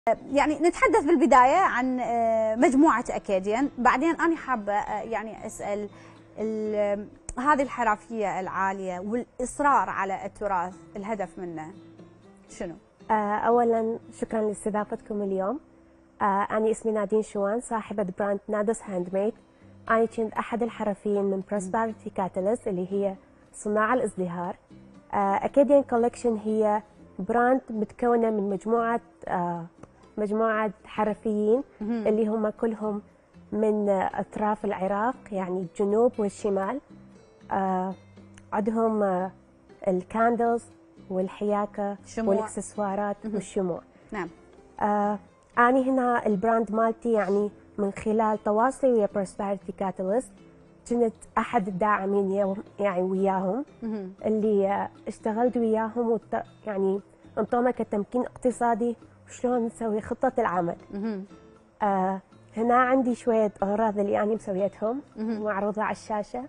الفنانة نادين شواني تتحدث عن بداياتها في صناعة الاكسسوارات
11258📺 افقي